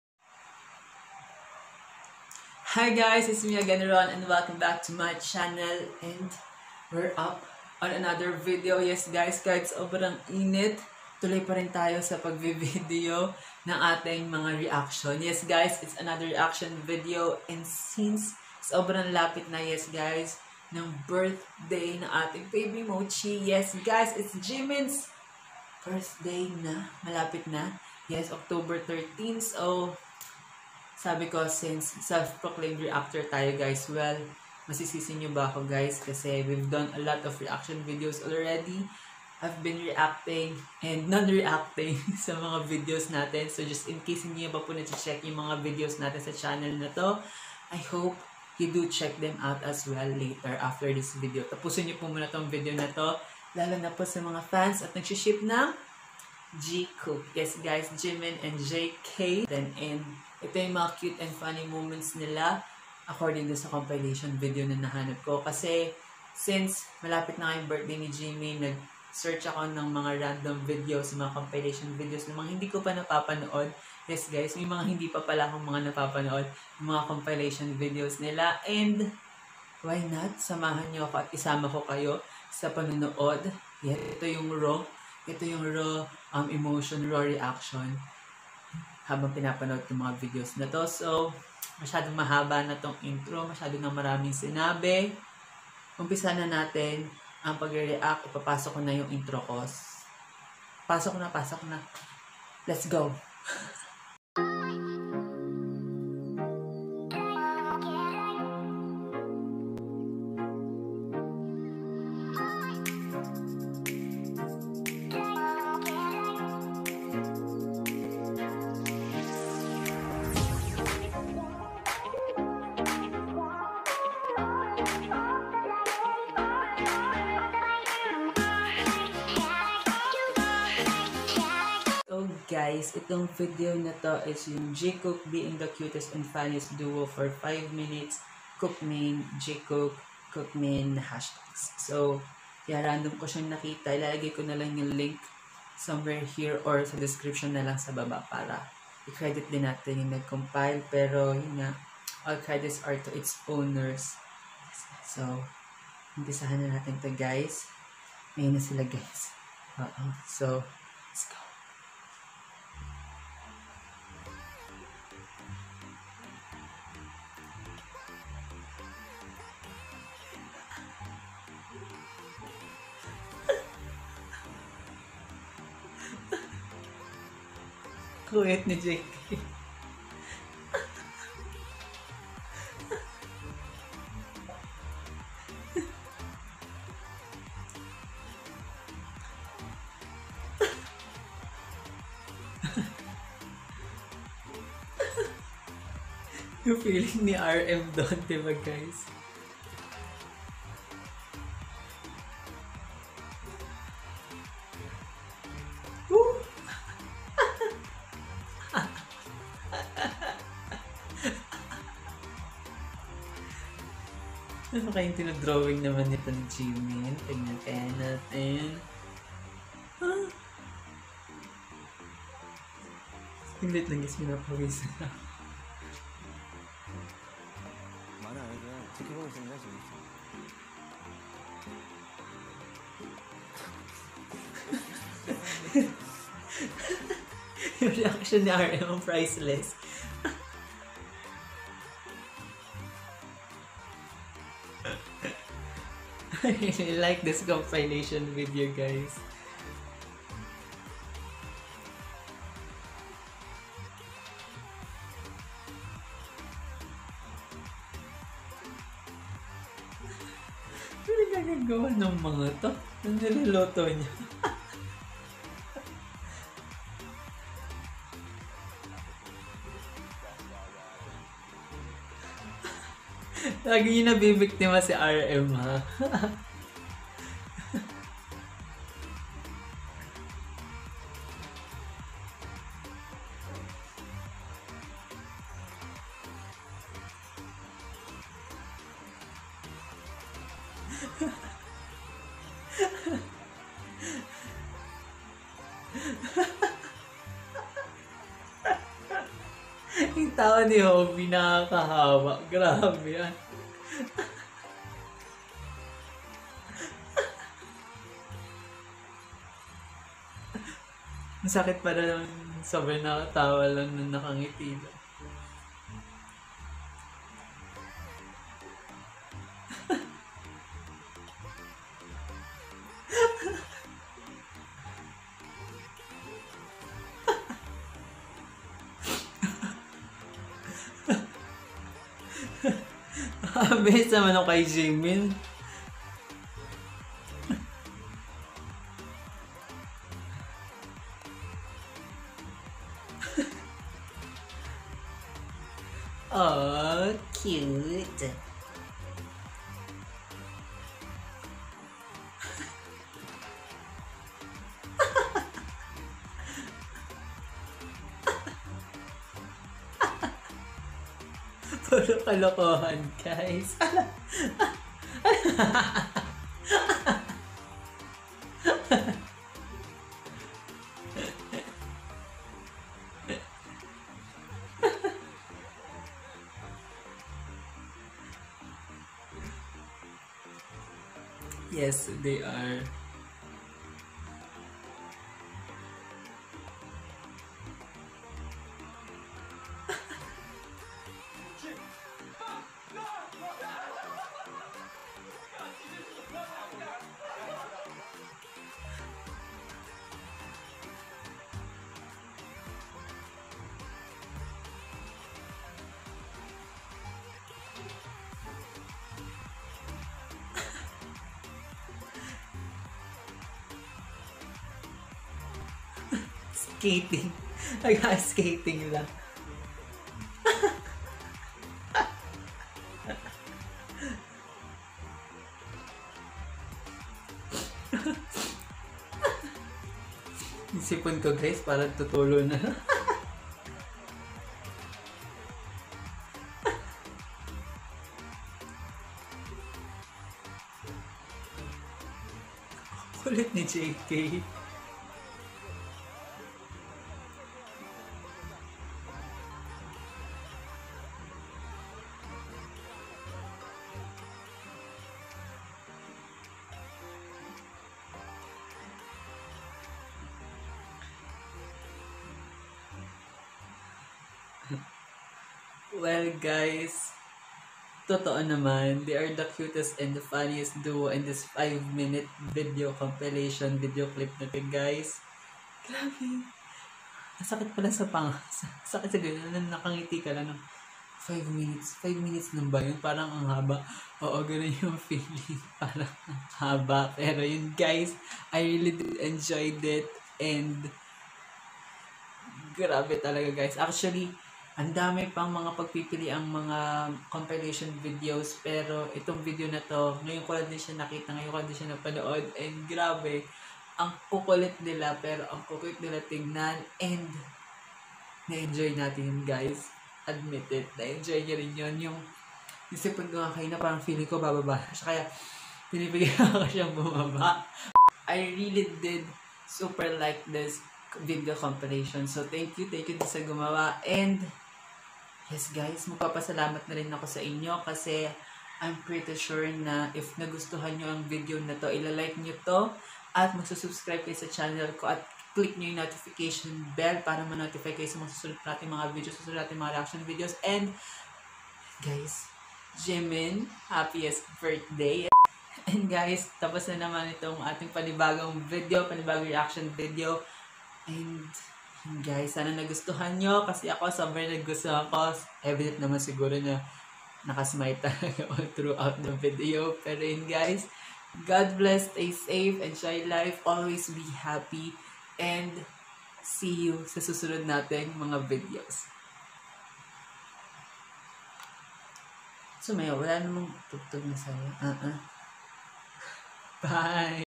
JIKOOK BEING THE CUTEST AND FUNNIEST DUO FOR 5 MINUTES REACTION VIDEO
Hi guys since I super loved Jimin and Jungkook, I tried to randomly search for a cute video to react to. And here I found this video from Live What You love youtube page.

Camera: Vivo S1
Edit: Vivo S1 and Filmorago
Thumbnail: Pizap App